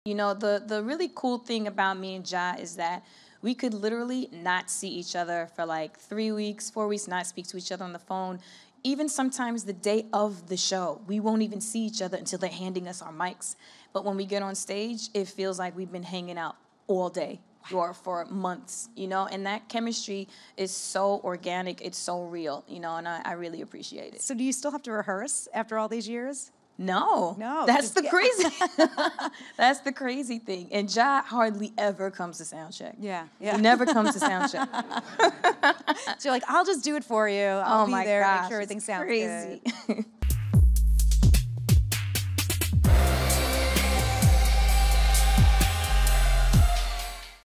What Working With Ja Rule Is Like For Ashanti
Ashanti talks about what it was like working with Ja Rule throughout the years.